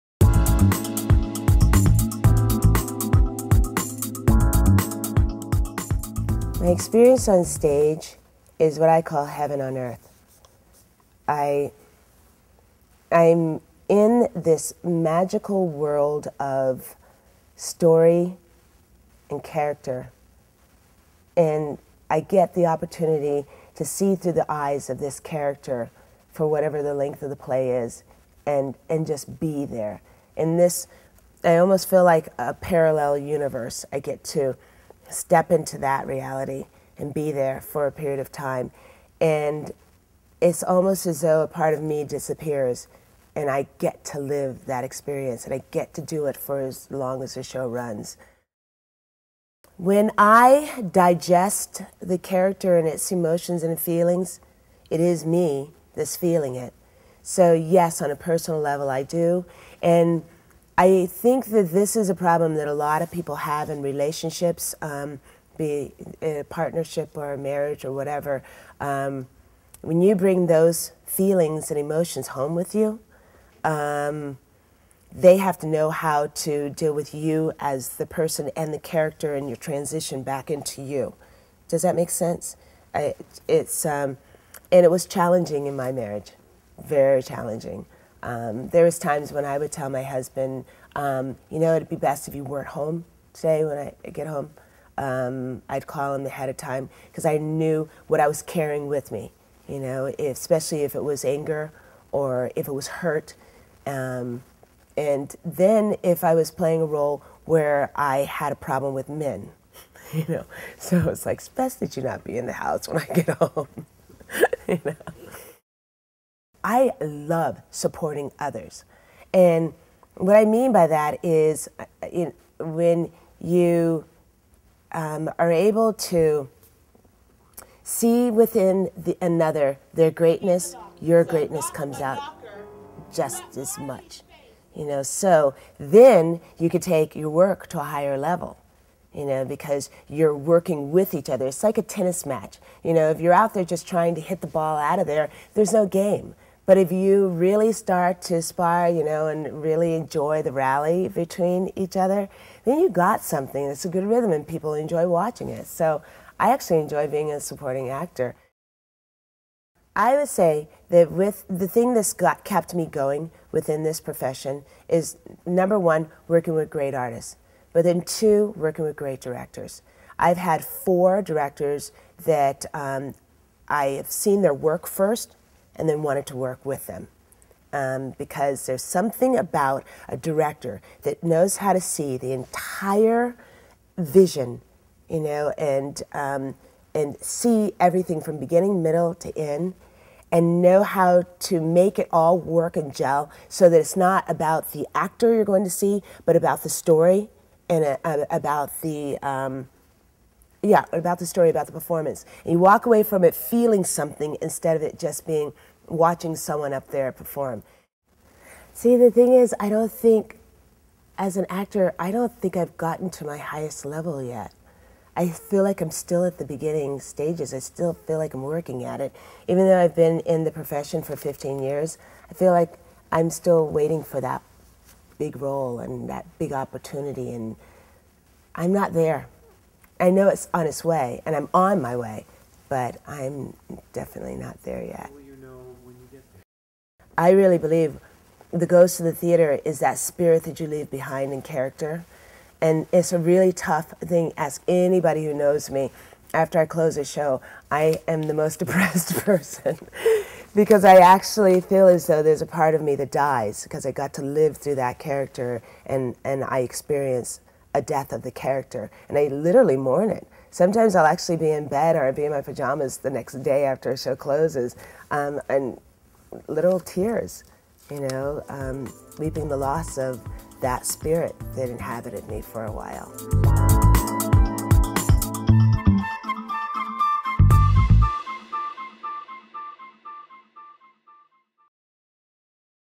Actor Spotlight: Lillie Richardson
Actress Lillie Richardson gives great interview about acting. Produced by Gene Ganssle Productions for Actor's Theatre.